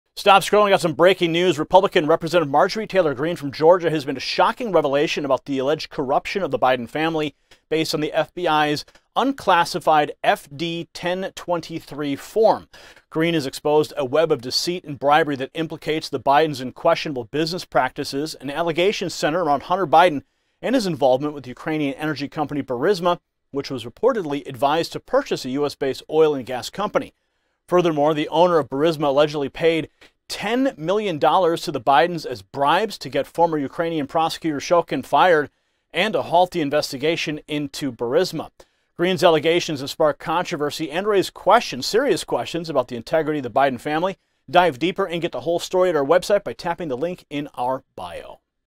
MT Greene Unveils FBI's FD-1023 Form Secrets
Stop scrolling! We have some breaking news! Republican Representative Marjorie Taylor Greene from Georgia has made a shocking revelation about the alleged corruption of the Biden family. Based on the FBI's unclassified FD-1023 form, Greene has exposed a web of deceit and bribery that implicates the Bidens in questionable business practices. The allegations center around Hunter Biden and his involvement with the Ukrainian energy company Burisma, which was reportedly advised to purchase a U.S.-based oil and gas company. Furthermore, the owner of Burisma allegedly paid $10 million to the Bidens as bribes to get former Ukrainian prosecutor Shokin fired and to halt the investigation into Burisma. Greene's allegations have sparked controversy and raise serious questions about the integrity of the Biden family. Dive deeper and get the whole story at our website. Tap the link in our bio.